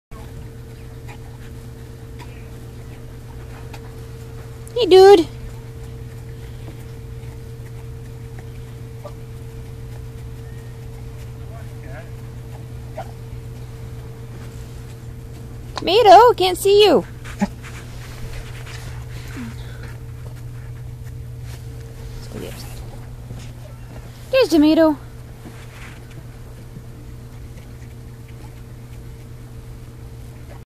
TURKEY DINNER!
We had 5 but these are the last two we have. Boy has no name but I named the girl Tomato. ^_^